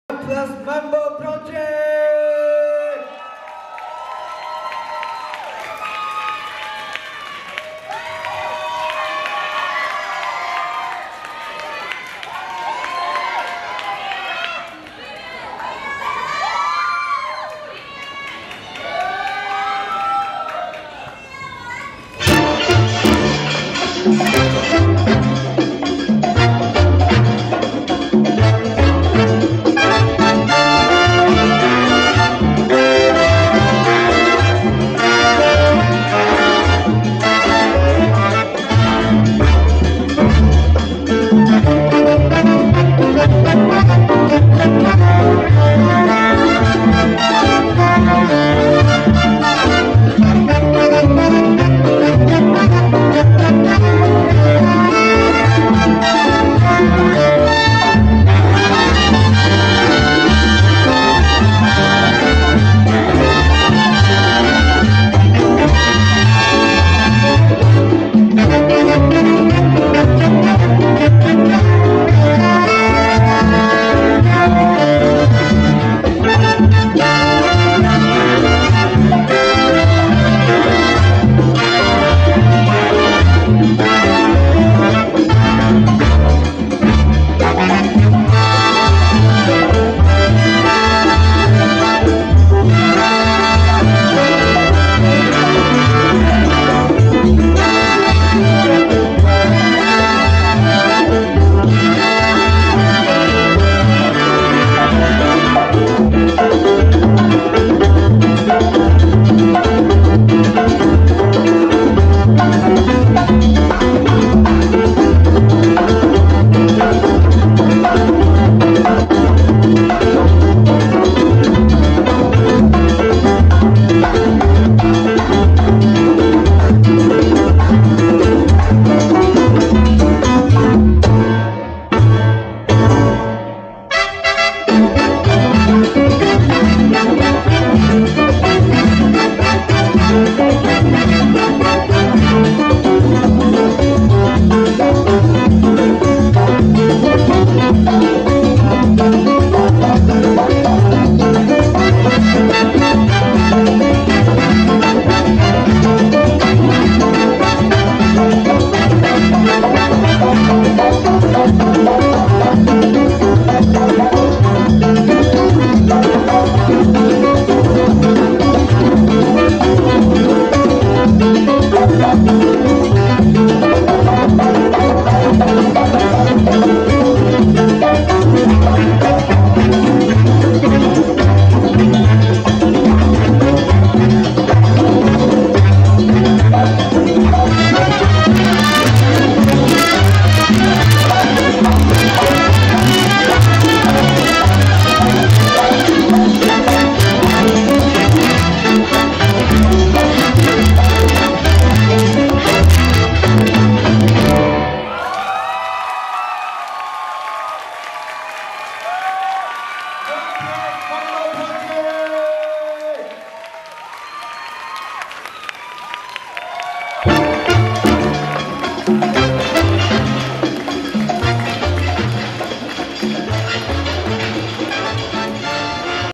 OnePlus Mambo Project @ Formosa Salsa Festival 2019
Video Record by Jason Ou
Cheoro by Yichia & Belle
Performed by Yichia, Belle, Nick, Catherine, Vivienne, Ali